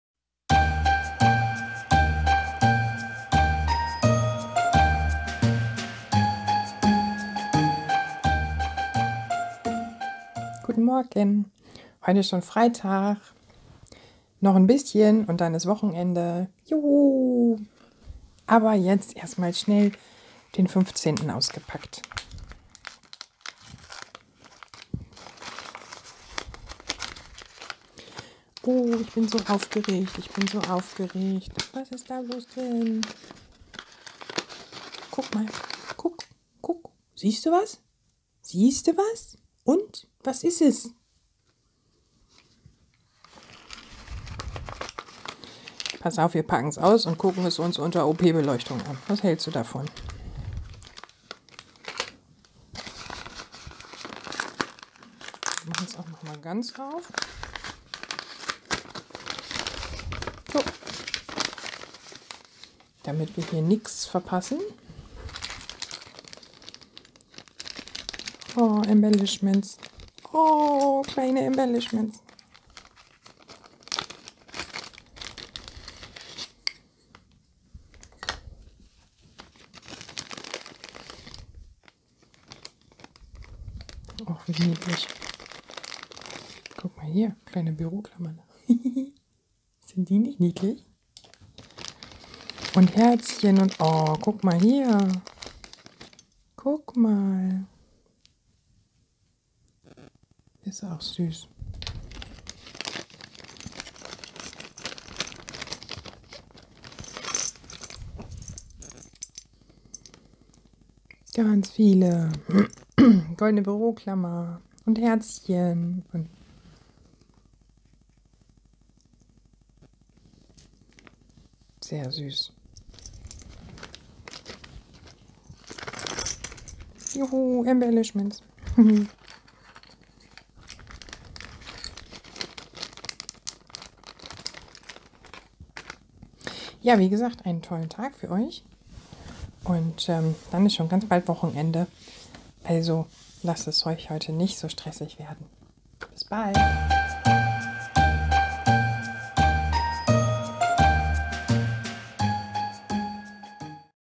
StempelPalast Adventskalender 15.12.2017
Ich öffne mit Euch meinen Adventskalender gefüllt mit Stampin' Up! Artikeln.
Meine Demokollegin Anja Ehmann hat ihn erstellt.